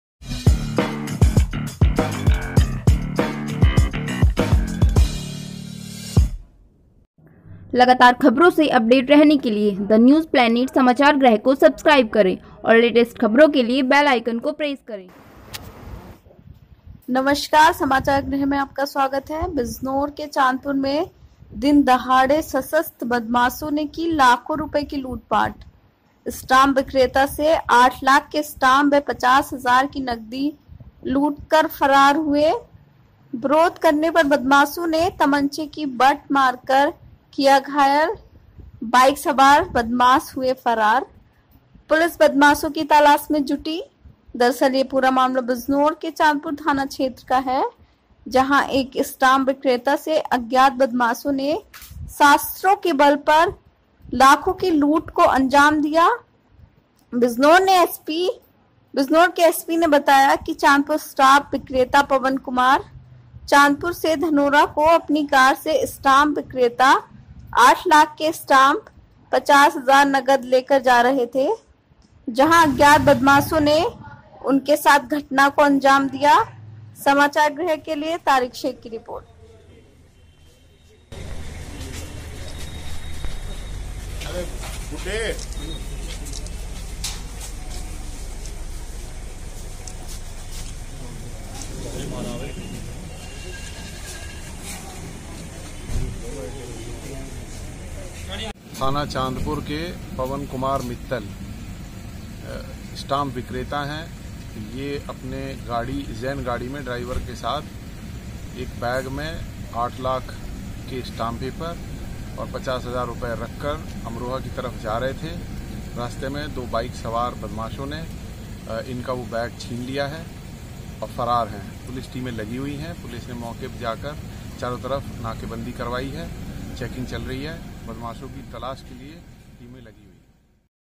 बिजनौर के चांदपुर में दिन दहाड़े बदमाशों ने कि लूट?
बिजनौर